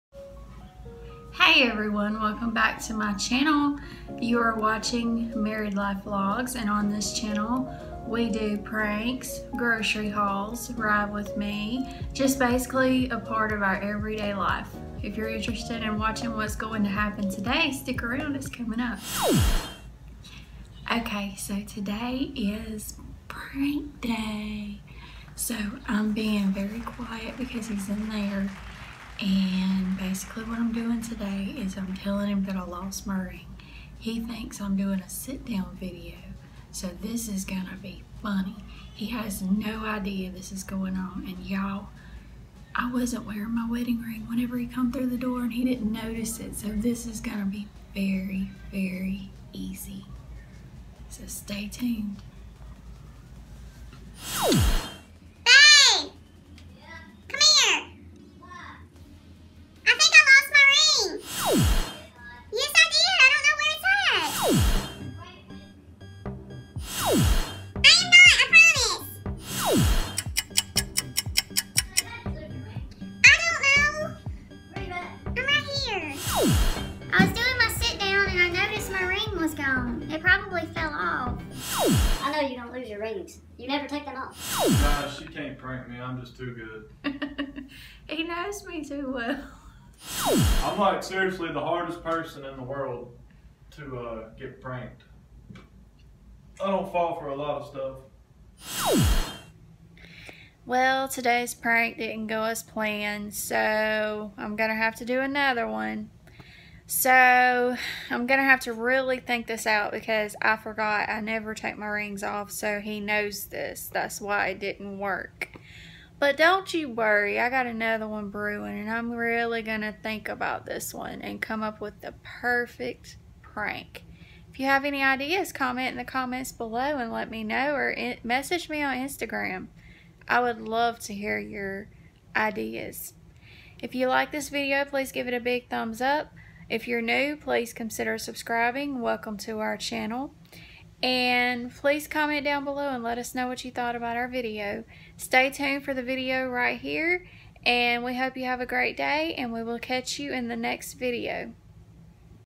Losing my wedding ring to see how my husband reacts * epic fail*
In this video I pranked my husband again did he fall for it or am I still losing tune in to find out!